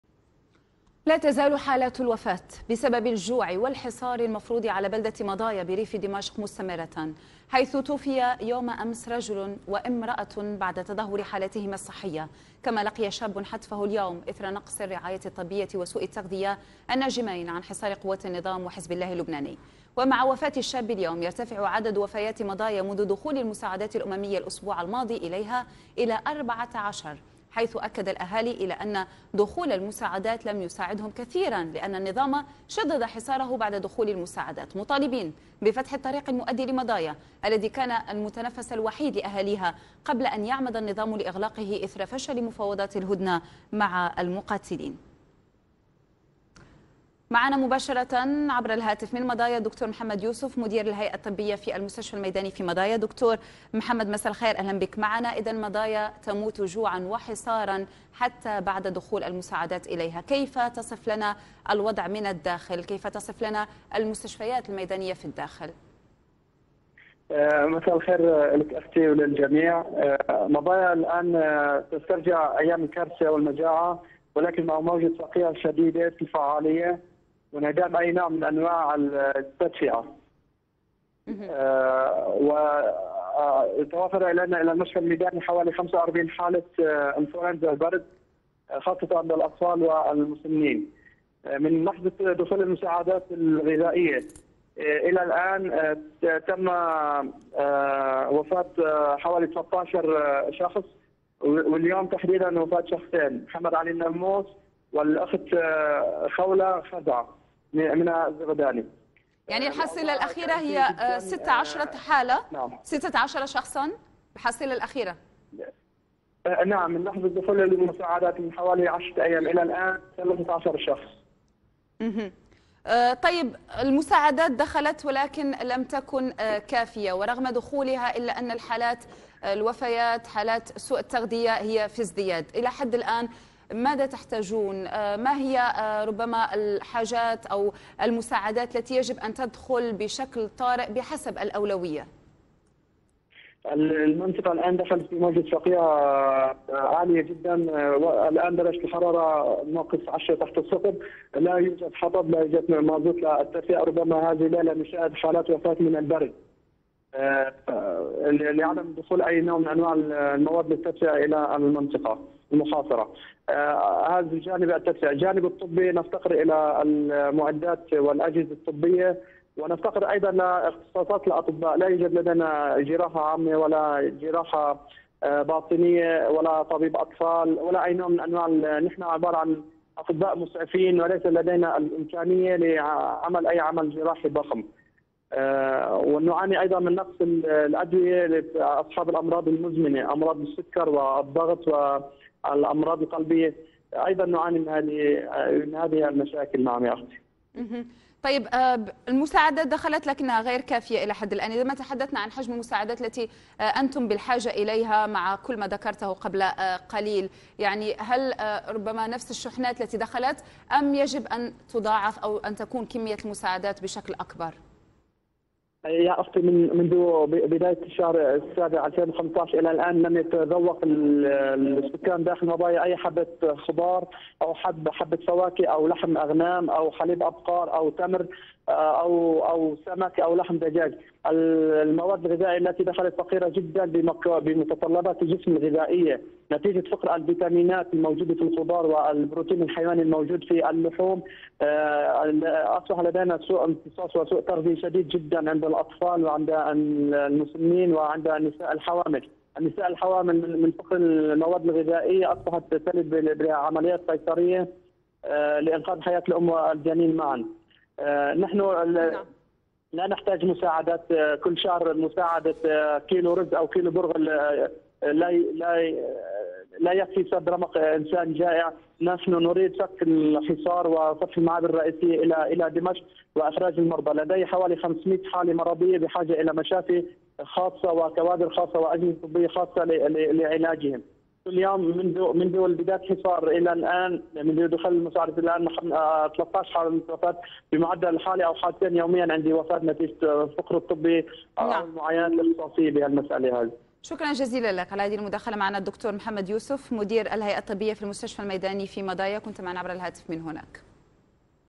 تواصل تسجيل وفيات إثر الجوع والمرض في مضايا المحاصرة
شاهد آيضا:
قيادي بعثي يفضح تاريخ آل الأسد!؟
لا تزال حالات الوفيات بسبب الجوع والحصار المفروض على بلدة مضايا بريف دمشق مستمرة، حيث توفي أمس رجل وإمراة بعد تدهور حالتهما الصحية، كما لقي شاب حتفه اليوم إثر نقص الرعاية الطبية وسوء التغذية الناجميْن عن حصار قوات النظام وحزب الله اللبناني.

ومع وفاة الشاب اليوم، يرتفع عدد وفيات مضايا منذ دخول المساعدات الأممية الأسبوع الماضي إليها إلى أربعة عشر، حيث أكد الأهالي أن دخول المساعدات لم يساعدهم كثيرا، لأن النظام شدد حصاره بعد دخول المساعدات، مطالبين بفتح الطريق المؤدي لمضايا والذي كان المتنفس الوحيد لأهاليها قبل أن يعمد النظام لإغلاقه إثر فشل مفاوضات الهدنة مع المقاتلين.